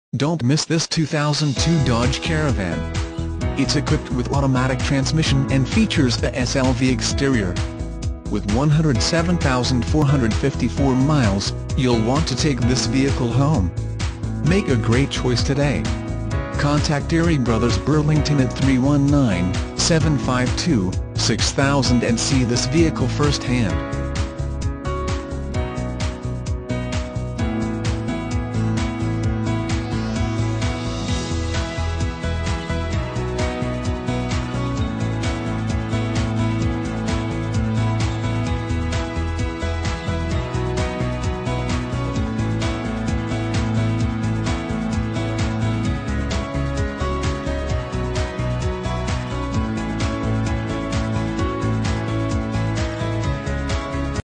2002 Dodge Caravan - Mini-van, Passenger West Burlington IA
2002 Dodge Caravan EL

Don't miss this 2002 Dodge Caravan. It's equipped with Automatic transmission and features a SLV exterior. With 107454 miles, You'll want to take this vehicle home. Make a great choice today.